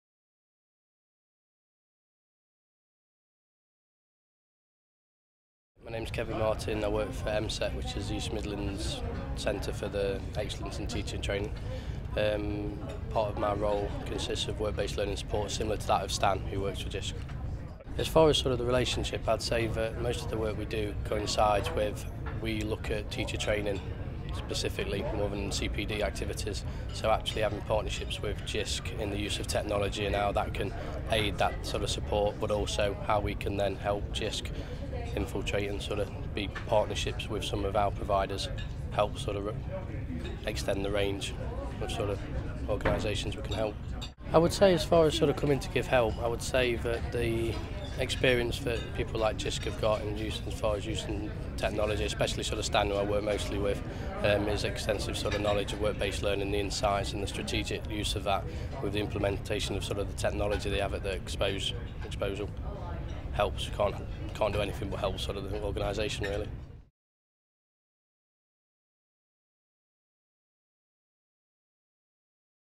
Working with partners
Filmed at the RSC East Midlands 2011 e-fair: "Becoming an agile learning provider" -- more details